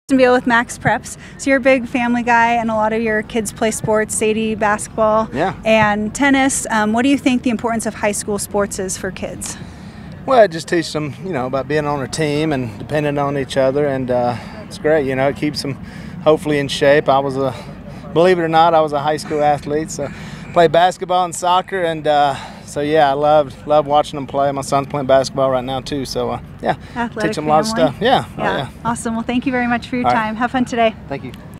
Interview with Willie Robertson of Duck Dynasty
Kirstin Beal talks with Willie Robertson of Duck Dynasty about the importance of high school sports.